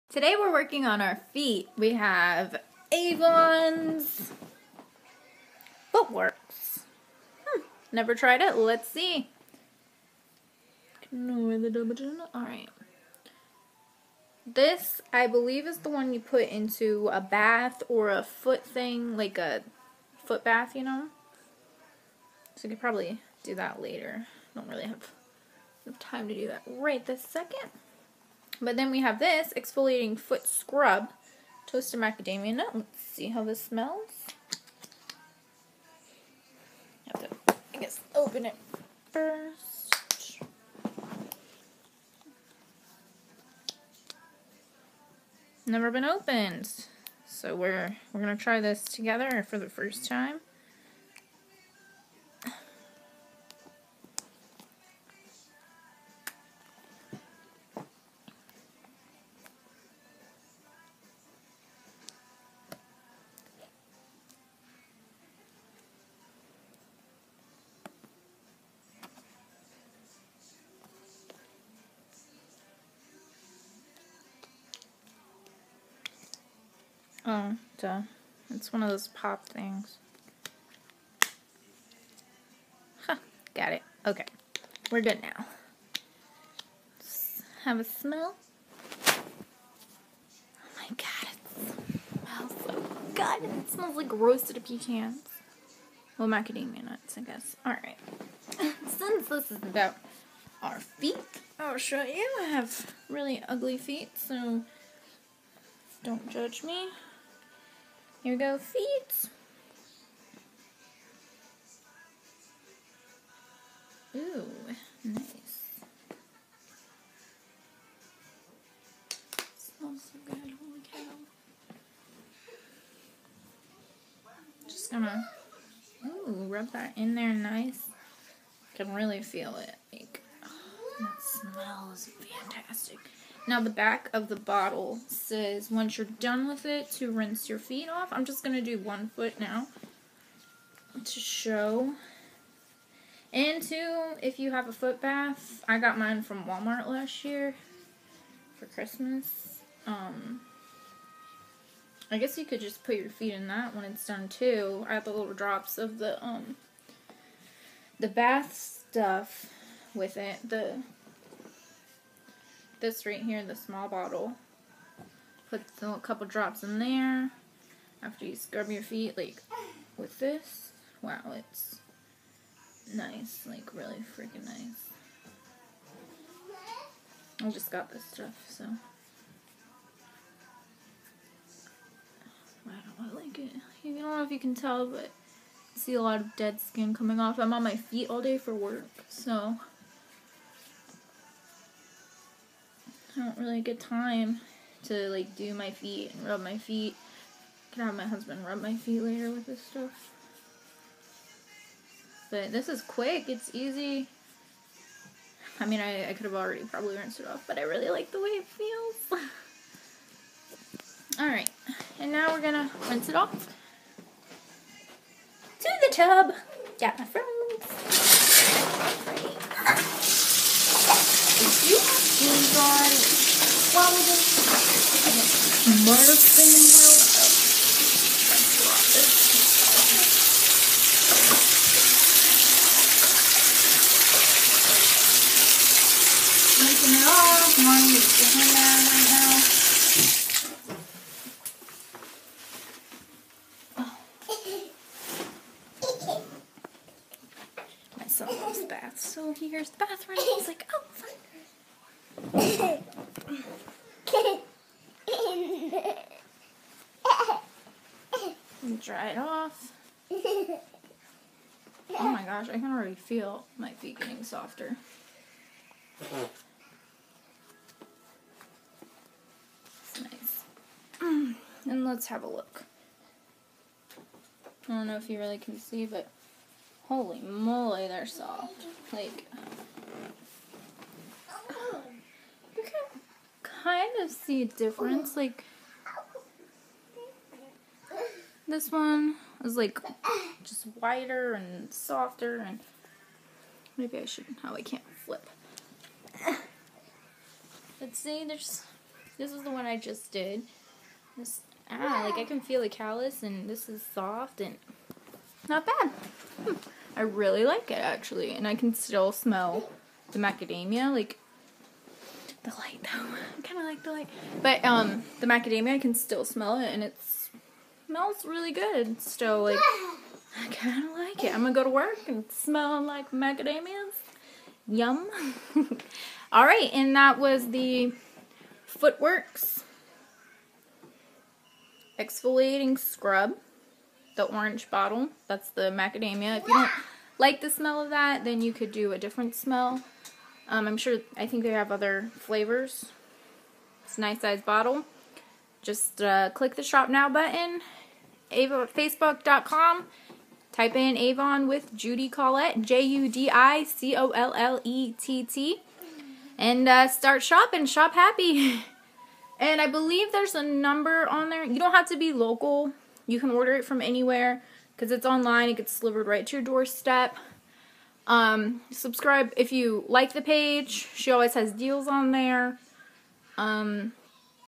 Foot works. Facebook search Avon with Judi Collett
Review of the Avon product foot works. To get yours visit Facebook.com and search Avon with Judi Collett